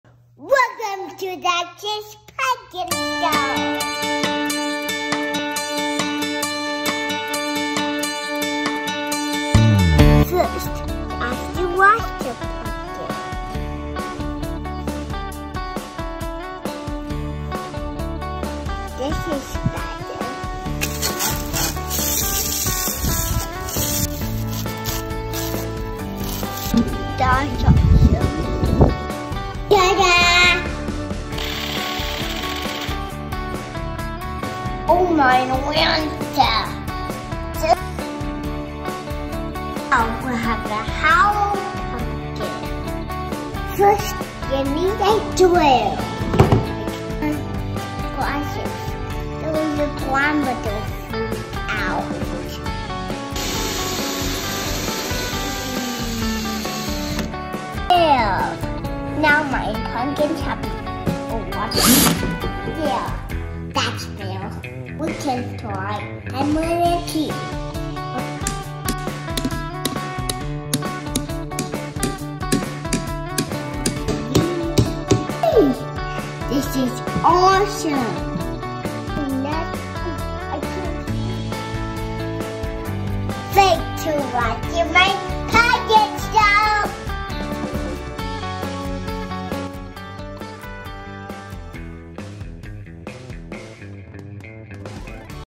Dax's 1st Pumpkin Show
Dax wanted to make a show about his first time making a pumpkin, so here it is!